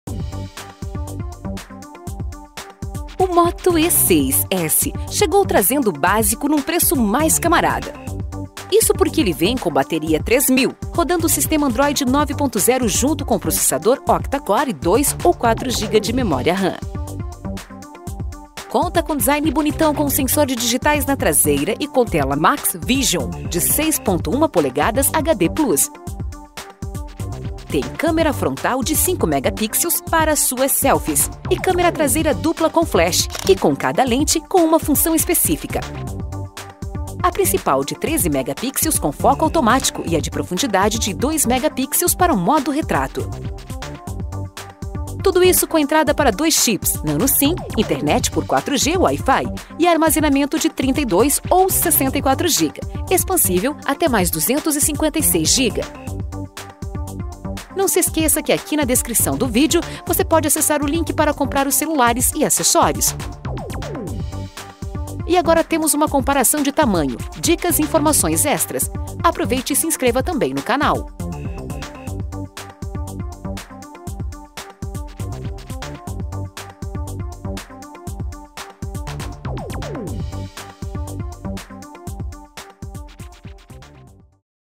Moto E6s (64 GB)
Ficha técnica em vídeo do Moto E6s (64 GB). Resumo em 1 minuto.

Link para comprar (Moto E6s)

O Moto E6s chegou trazendo o básico num preço mais camarada.

Isso porque ele vem com bateria 3000 mAh, rodando o sistema android 9.0 junto com processador Octa Core e 2 GB ou 4 GB de memória RAM.
Conta com design bonitão com sensor de digitais na traseira, e com tela Max Vision de 6.1 polegadas HD Plus.

Tem câmera frontal de 5 megapixels para suas selfies. E câmera traseira dupla com flash, e com cada lente com uma função específica.

A principal de 13 megapixels com foco automático, e a de profundidade de 2 megapixels para modo retrato.

Tudo isso com entrada para dois chips Nano Sim, internet por 4G ou Wi-Fi, e armazenamento de 32 GB ou 64 GB. Expansível até mais 256 GB.

Especificações:
Sistema operacional Android 9
Memória (RAM) 2 GB ou 4 GB
Armazenamento (ROM): 32 GB ou 64 GB
Armazenamento disponível: 21 GB e 51 GB
Tela Max Vision de 6.1" HD+ (1560 x 720 pixels)
Bandas 2G: GSM banda 2/3/5/8
3G: WCDMA banda 1/2/4/5/6/8
4G: LTE banda 1/2/3/4/5/7/8/19/28
Conteúdo da caixa
01 Telefone
01 Capa Protetora
01 Kit de Manuais
01 Fone de Ouvido Estéreo
01 Cabo de Sincronização
01 Carregador de Parede
01 Ferramenta de Remoção do Chip
Dimensões Altura (mm): 155.6
Largura (mm): 73.0
Profundidade (mm): 8.5
Conectividade micro USB, entrada de fone de ouvido de 3.5 mm
NFC Não
Câmera traseira 13 MP + 2 MP com auto foco
Lente 80º - Abertura f/2.2 (13 MP)
Lente 90º - Abertura f/2.4 (2 MP)
Flash LED
Cartão SIM Dual Nano Chip + SD Card
Wi-Fi Sim
TV Digital Não
Tecnologia Bluetooth® 4.2
Captura de vídeo FHD@30fps, FHD@60fps, HD@30fps
Câmera frontal 5 MP; f2.2; 77°, flash de tela
Google Fotos Sim
Componentes do sistema/processador Helio P22, 2.0 GHz Octa-Core
Bateria 3.000 mAh
Serviços de localização GPS, AGPS, LTEPP, SUPL, Glonass
Sensores:
Leitor de impressão digital
Sensor de proximidade
Acelerômetro
Sensor de luz ambiente
Giroscópio